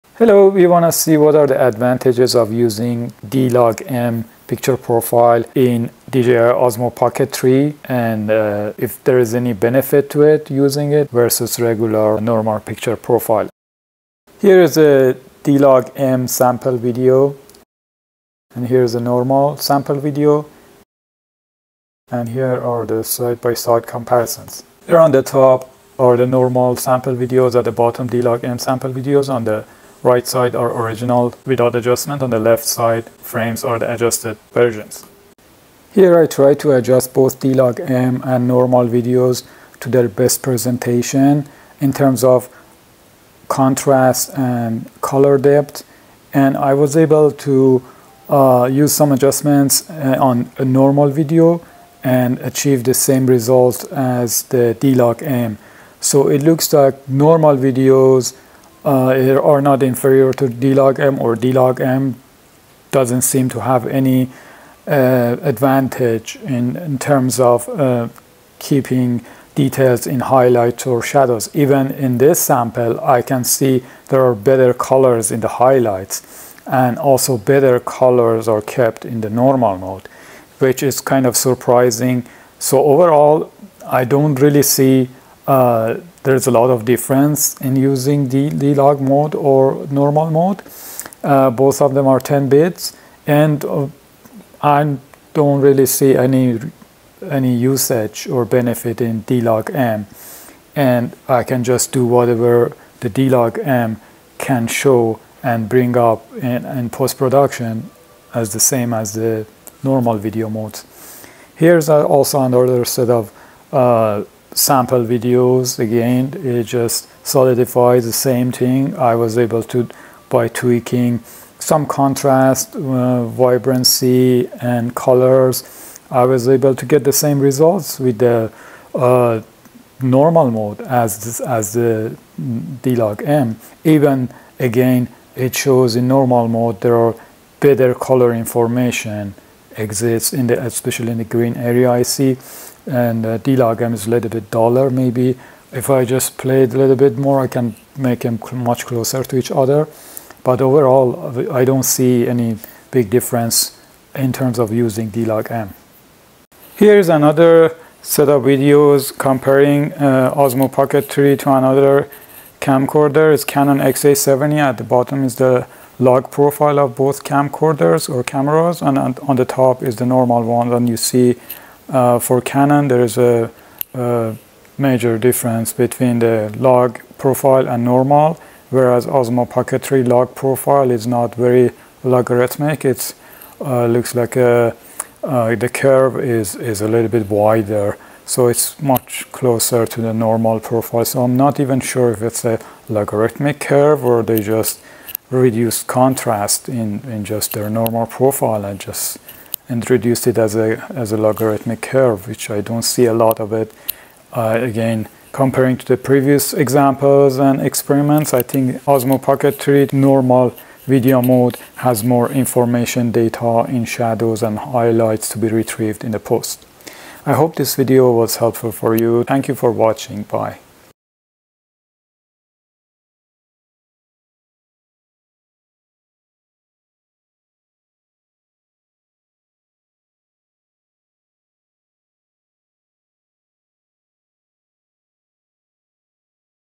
D-Log M Comparison With Normal | DJI Osmo Pocket 3 | Canon XA70
In this video I demonstrate some experiment in comparing videos shot in D-Log M versus videos sot in Normal mode.